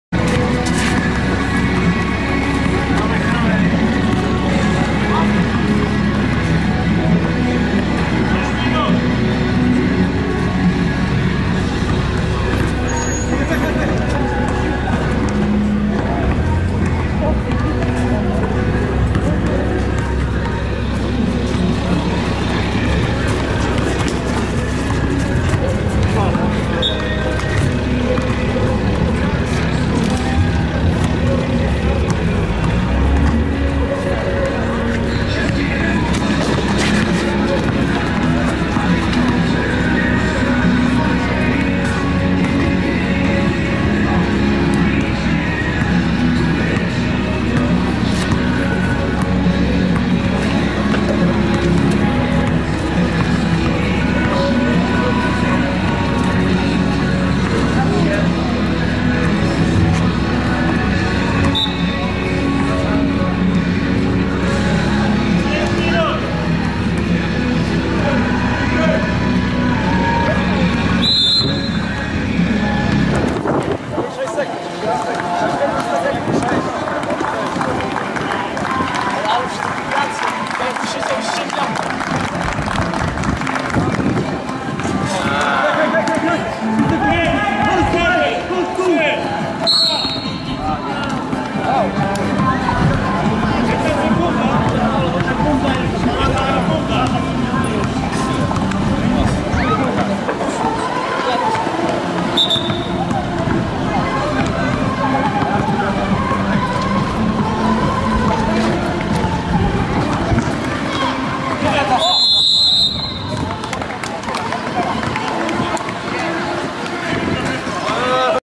streetball finał video
W sobotę, 28 czerwca na Placu Polonii Amerykańskiej w Brzegu odbył się XIX Streetball Brzeski - turniej koszykówki ulicznej. Fragment meczu finałowego pomiędzy GTW a Gramy Bez Spiny. Wygrana GTW 7:6.